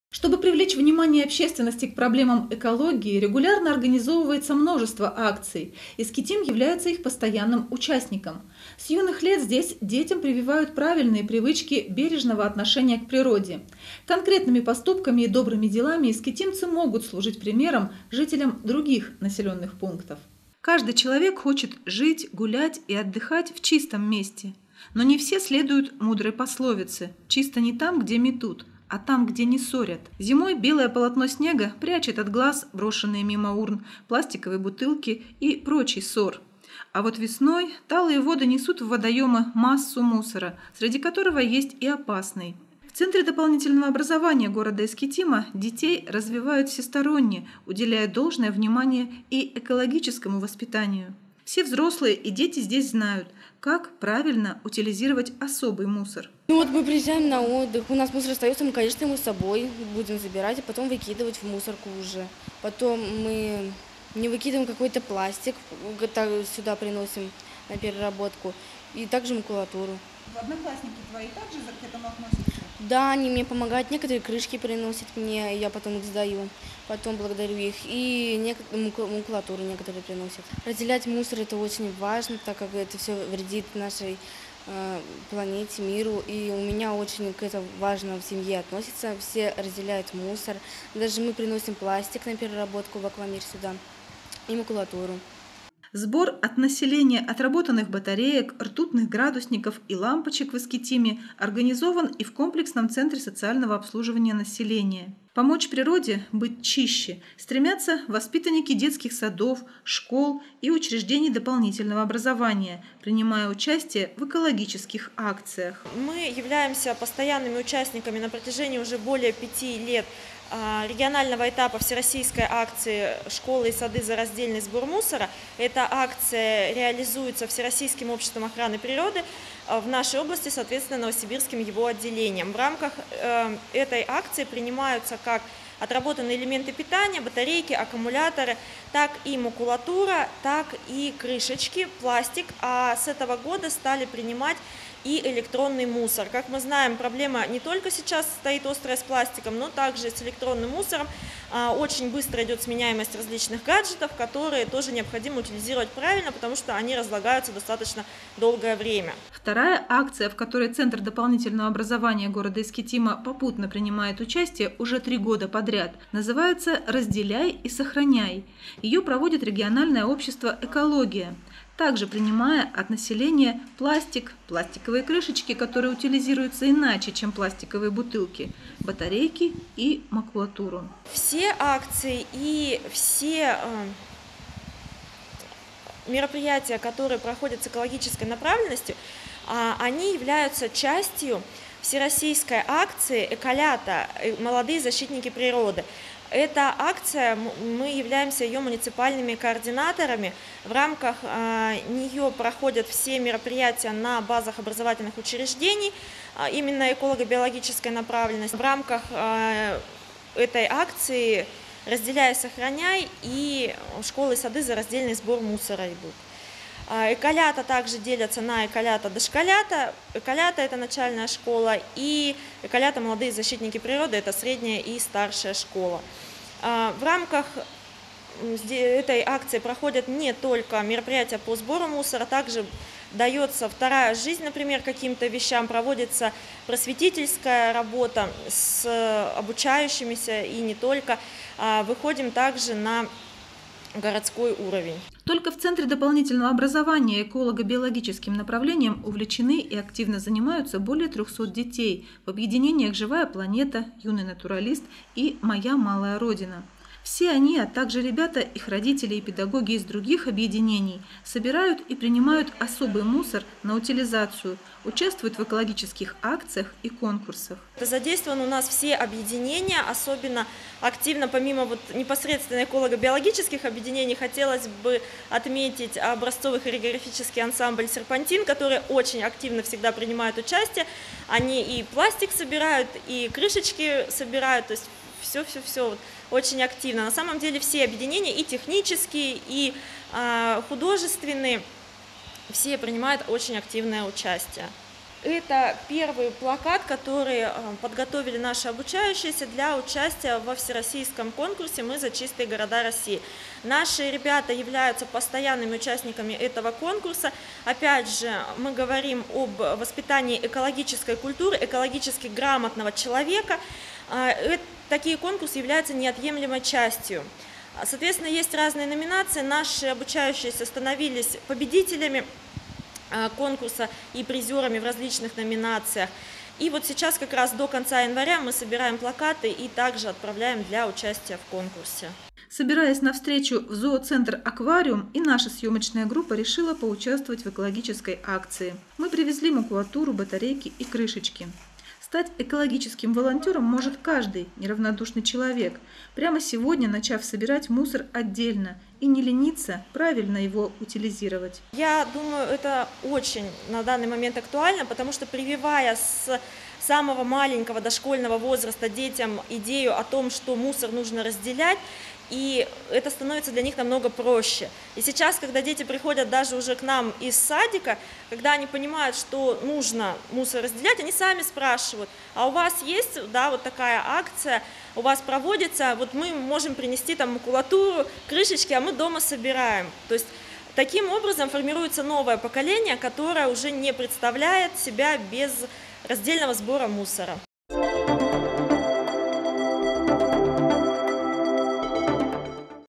Экоакция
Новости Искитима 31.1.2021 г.
Не выбрасывать в природу опасный мусор и перерабатывать отдельное сырье вторично призывают экологические акции и конкурсы, в которых активно участвуют искитимские дети.